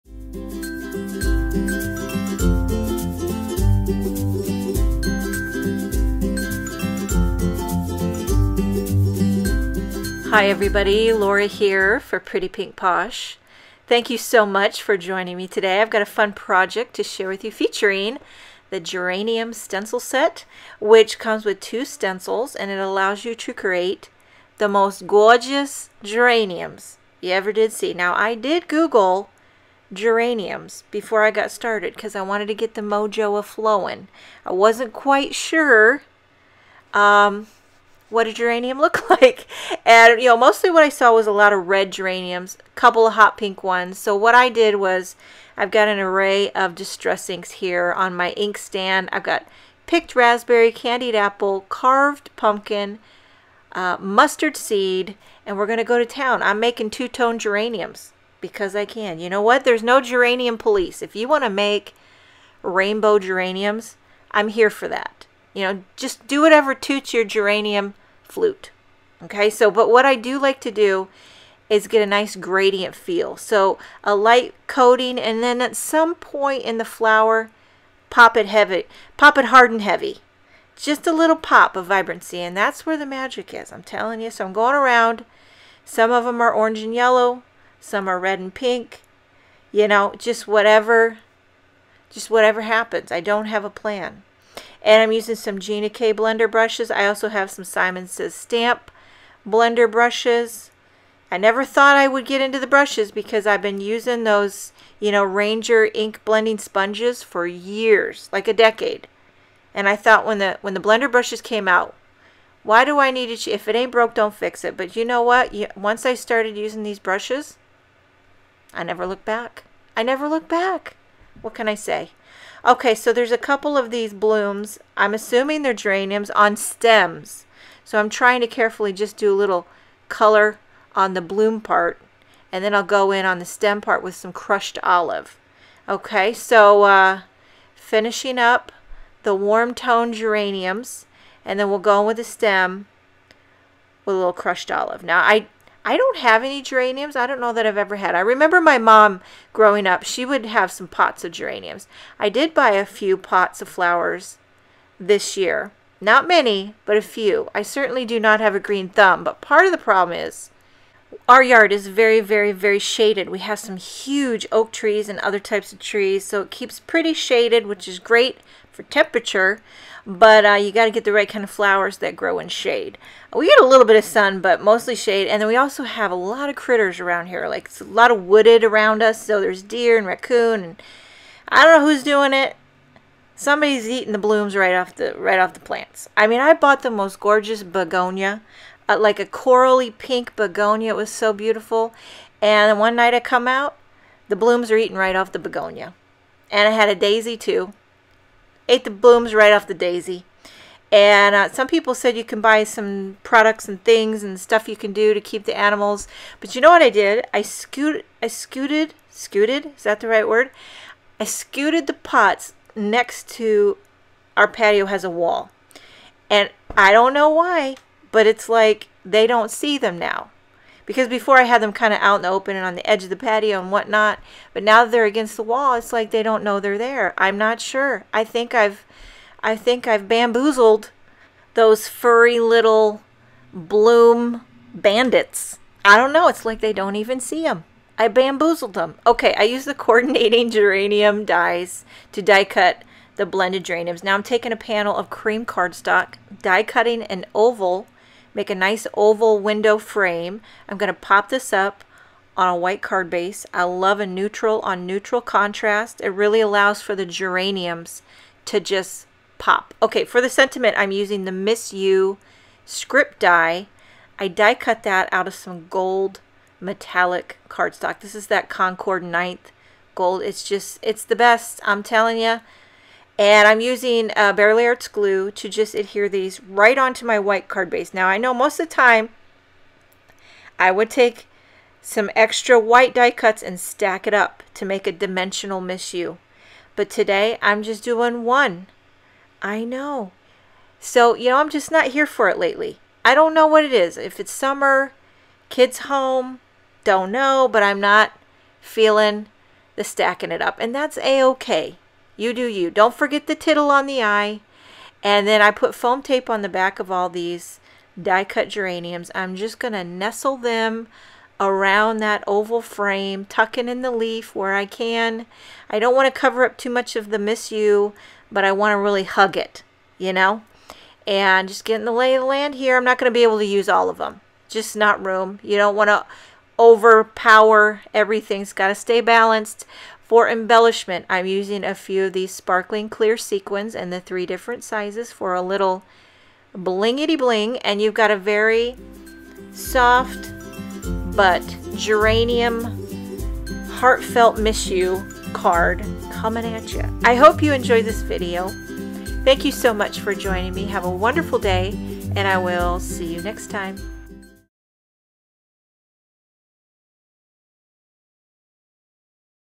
Geranium Miss You with Laura Bassen
We are thrilled to have @laurafadora1 sharing a beautiful card combining our Geraniums stencils and dies. The perfect way to let someone know you miss them!

Links to the main products below and more details, photos and links in the PPP blog post:

Visit Pretty Pink Posh for more card ideas, videos and inspiration